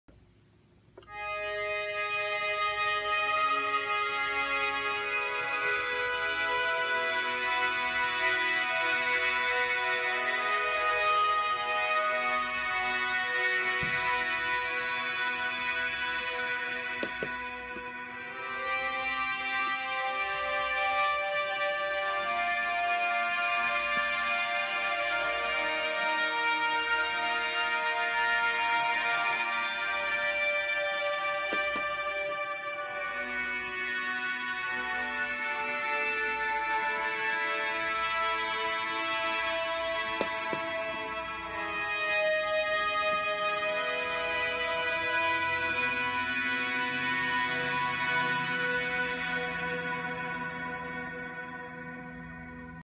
Adaptive Bluetooth Joystick Synth
I have been building joystick musical instruments for people of any ability. Here is the most recent.